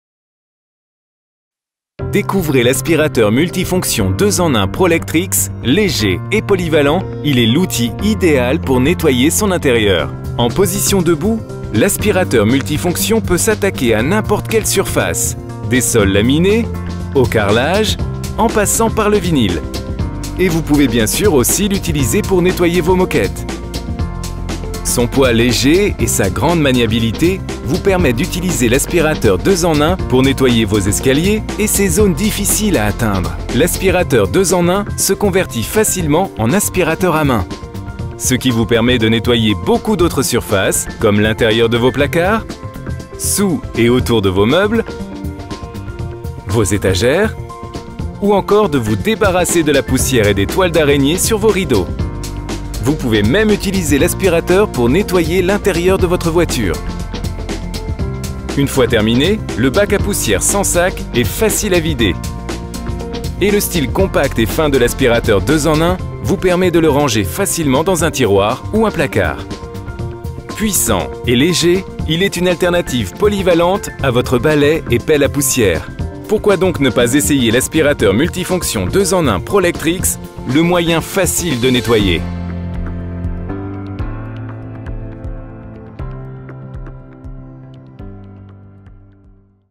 l’aspirateur multifonction 2 en 1 Prolectrix
Découvrez l’aspirateur multifonction 2 en 1 Prolectrix ; léger et polyvalent, il est l’outil idéal pour nettoyer son intérieur